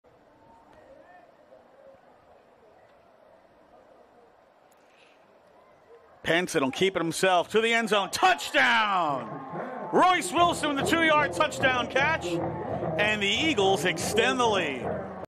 Santa Fe vs Willowridge BGC Houston Football - Week 11, 2023
2 YD Touchdown catch for the Eagles!